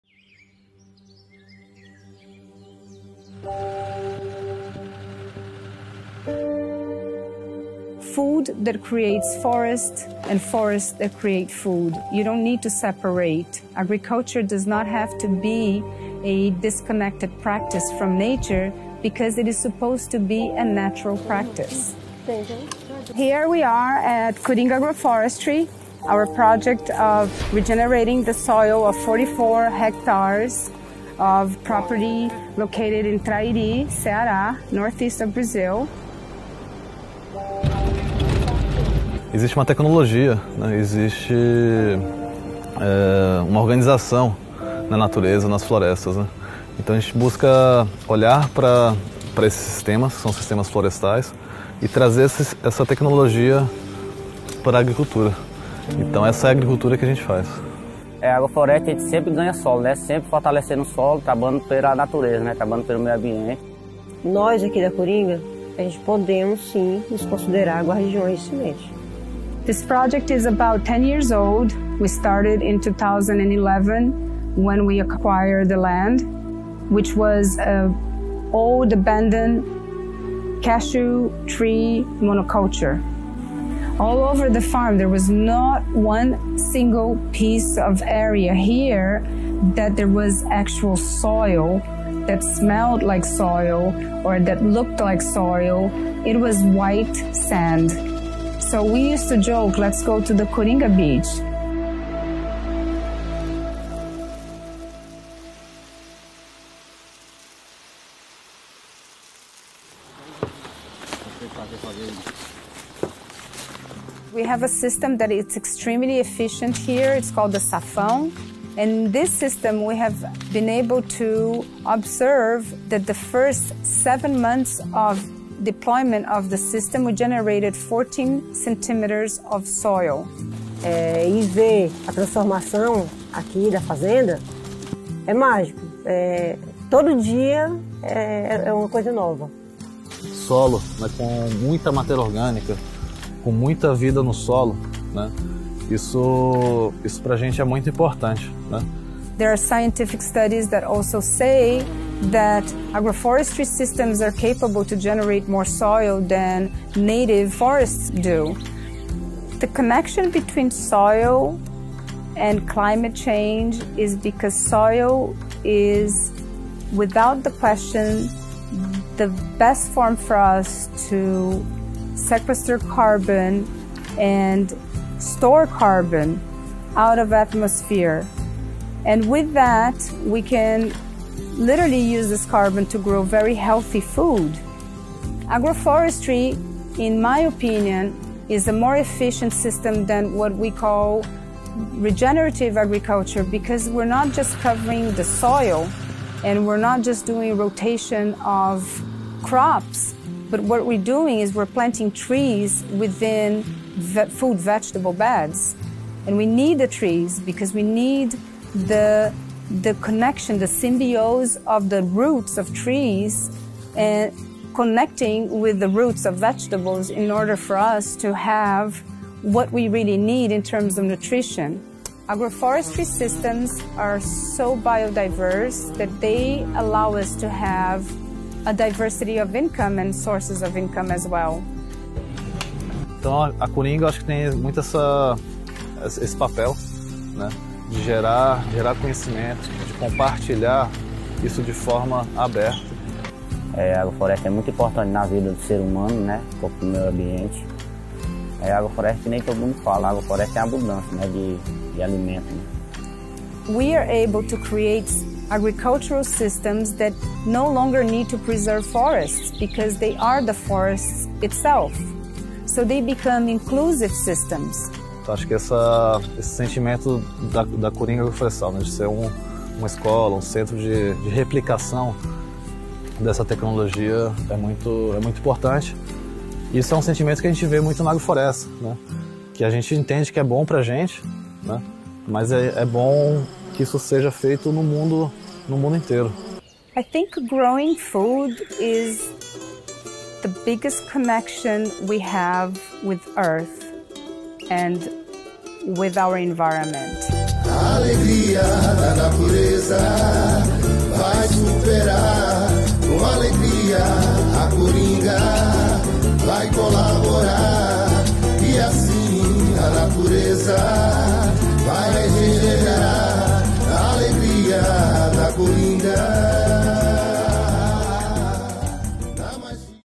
Coringa Agrofloresta institutional film
Soil offers a major chunk of the world’s actual and potential carbon storage. Increasing carbon in soil is an effective way of removing dangerous levels of carbon dioxide in the atmosphere, as well as providing benefits such as increased and more resilient food production. Soil carbon can help tackle climate change, and quality soil helps in the production of food without the necessity of synthetic fertilizers, pesticides and herbicides.
Agroforestry is the solution for food security, sustainable development, biodiversity conservation and economic growth.

In this film that our friend @chance_foreman made for the our website and the first film we publish in both English and Portuguese, we share a bit about our field work and efforts regenerating soil .

We would like to thank everyone who support our journey. Wish you all a good weekend.

The revolution will be agroforestry 🌱🌳💚

**Versão em português

O solo oferece uma parte importante do armazenamento de carbono real e potencial do mundo. Aumentar o carbono no solo é uma forma eficaz de remover níveis perigosos de dióxido de carbono na atmosfera, além de proporcionar benefícios como o aumento e a resistência da produção de alimentos. O carbono do solo pode ajudar a combater as mudanças climáticas, e o solo de qualidade ajuda na produção de alimentos sem a necessidade de fertilizantes sintéticos, pesticidas e herbicidas.
A agrofloresta é a solução para segurança alimentar, desenvolvimento sustentável, conservação da biodiversidade e crescimento econômico.

Neste filme que nosso amigo @chance_foreman fez para o nosso site e o primeiro filme que publicamos em inglês e português, compartilhamos um pouco sobre nosso trabalho de campo e esforços de regeneração do solo.

Gostaríamos de agradecer a todos que apoiam nossa jornada. Desejamos a todos um bom fim de semana.

A revolução será agroflorestal 🌱🌳💚